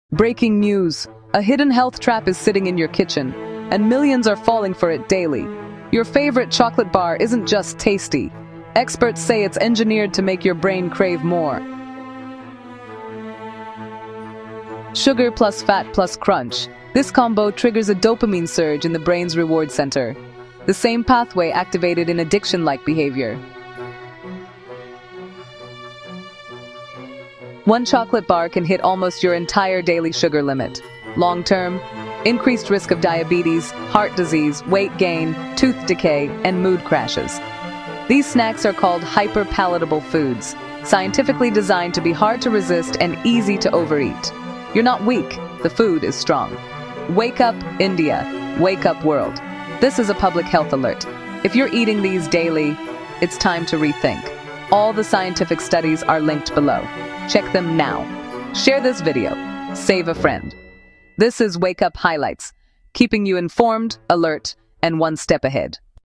4 Hidden Dangers of Eating Chocolate Every Day
Your favourite chocolate bar may be more dangerous than you think.
Experts warn that the sugar–fat–salt combo in popular snacks creates hyper-palatable foods — engineered to trigger dopamine spikes and addiction-like behaviours.
Over time, daily consumption is linked to diabetes, heart disease, weight gain, inflammation, mood swings, and strong cravings.

This is a Wake Up Highlights Public Health Alert.
Read the studies below and protect yourself and your family.

Avena et al. — Evidence for sugar addiction

Fazzino et al. — Hyper-palatable foods research